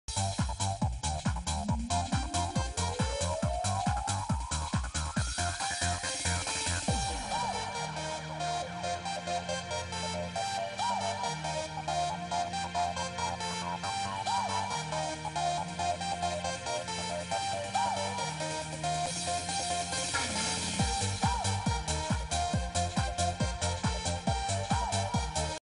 الإتحاد - التحدي 2-0 ( الإسبوع الثاني من الدوري الليبي 2013 / 2014 ) هدف كوفي
ليبيا ميديا